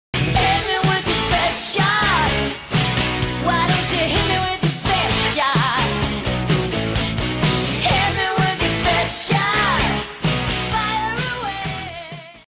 Team Llama nomenal gishwhes 2016 Item 5
In the middle of a mall food court, you and a friend (one or more) play a nice game of badminton - we must see the tennis whites, the net, rackets, etc.